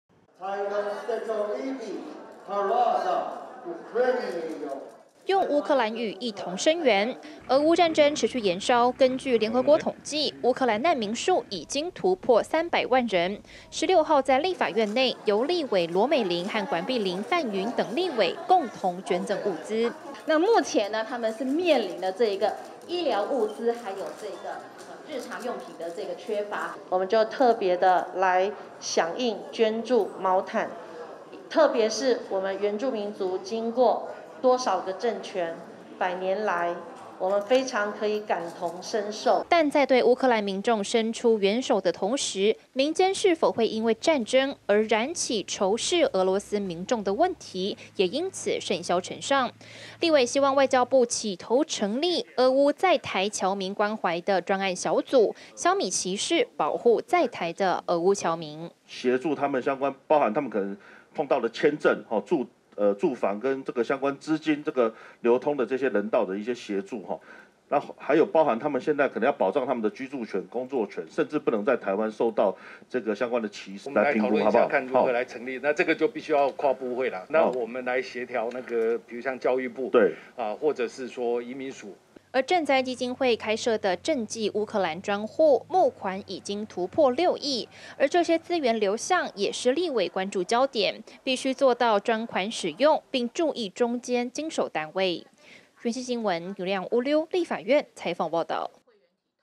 立委捐物資助烏克蘭 討論保障烏俄在台僑民｜每日熱點新聞｜原住民族電視台
俄烏戰爭持續延燒，國際間對俄羅斯祭出制裁的同時，也紛紛對難民伸出援手，今天(16)在立法院，多位立委也捐獻物資援助，而在外交委員會質詢上，就怕民間發生仇俄現象，因此如何保障在台俄烏僑民的安全，也成為討論焦點。而賑災基金會開設的賑濟烏克蘭專戶，募款已經突破6億，必須做到專款使用。